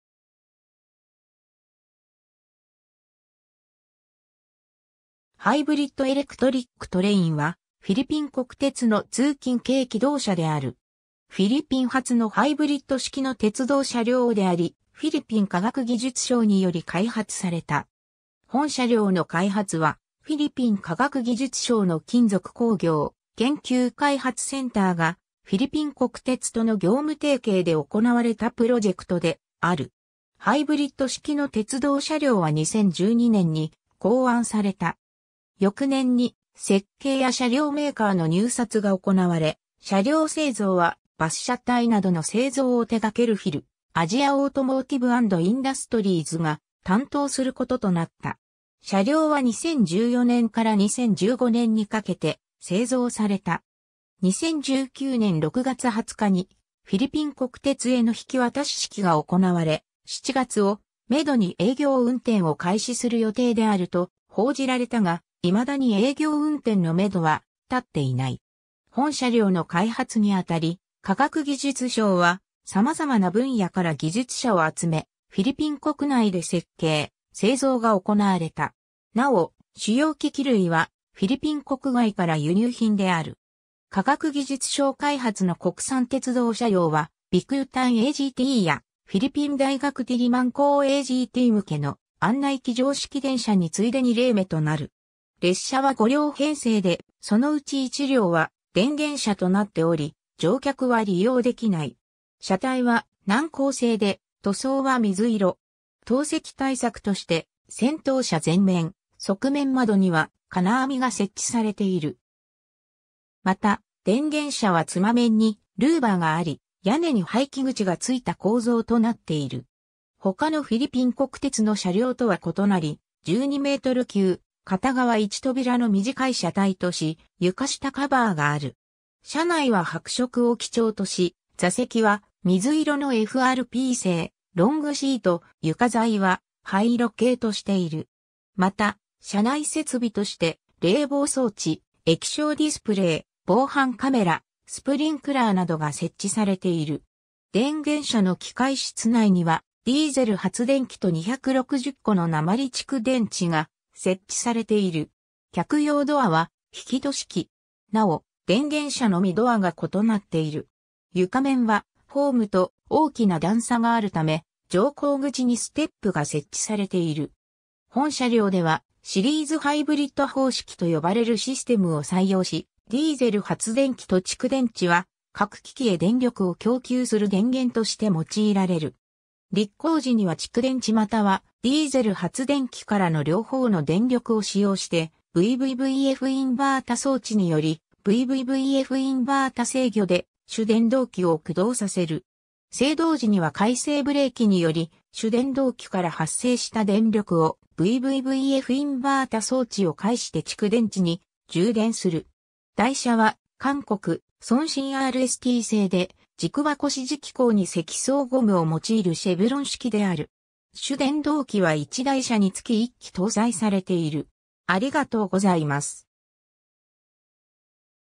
ハイブリッド・エレクトリック・トレイン
ハイブリッド・エレクトリック・トレイン（英: Hybrid Electric Train、略称HET）は、フィリピン国鉄の通勤形気動車である。
 フィリピン初のハイブリッド式（シリーズ方式）の鉄道車両であり、フィリピン科学技術省により開発された。
 本車両の開発は、フィリピン科学技術省の金属工業研究開発センターがフィリピン国鉄との業務提携で行われたプロジェクトである。
 ハイブリッド式の鉄道車両は2012年に考案された。
翌年に設計や車両メーカーの入札が行われ、車両製造はバス車体などの製造を手掛けるフィル-アジア・オートモーティブ&インダストリーズが担当することとなった。
車両は2014年から2015年にかけて製造された。
2019年6月20日にフィリピン国鉄への引き渡し式が行われ、7月をめどに営業運転を開始する予定であると報じられたが、未だに営業運転のめどは立っていない。
 本車両の開発にあたり、科学技術省は様々な分野から技術者を集め、フィリピン国内で設計・製造が行われた。
なお、主要機器類はフィリピン国外から輸入品である。
 科学技術省開発の国産鉄道車両は、ビクータンAGTやフィリピン大学ディリマン校AGT向けの案内軌条式電車に次いで2例目となる。
 列車は5両編成で、そのうち1両は電源車となっており、乗客は利用できない。
 車体は軟鋼製で、塗装は水色（下部は灰色の帯と白色）。
投石対策として先頭車前面、側面窓には金網が設置されている。
また、電源車は妻面にルーバーがあり、屋根に排気口が付いた構造となっている。
他のフィリピン国鉄の車両とは異なり、12m級・片側1扉の短い車体とし、床下カバーがある。
 車内は白色を基調とし、座席は水色のFRP製ロングシート、床材は灰色系としている。
また、車内設備として冷房装置、液晶ディスプレイ、防犯カメラ、スプリンクラーなどが設置されている。
電源車の機械室内には、ディーゼル発電機と260個の鉛蓄電池が設置されている。
 客用ドアは引き戸式。
なお、電源車のみドアが異なっている。
床面はホームと大きな段差があるため、乗降口にステップが設置されている。
 本車両では、シリーズハイブリッド方式と呼ばれるシステムを採用し、ディーゼル発電機と蓄電池は各機器へ電力を供給する電源として用いられる。
 力行時には蓄電池またはディーゼル発電機からの両方の電力を使用して、VVVFインバータ装置によりVVVFインバータ制御で主電動機を駆動させる。
制動時には回生ブレーキにより主電動機から発生した電力をVVVFインバータ装置を介して蓄電池に充電する。
 台車は韓国・ソンシンRST製で、軸箱支持機構に積層ゴムを用いるシェブロン式である。
主電動機は1台車につき1基搭載されている。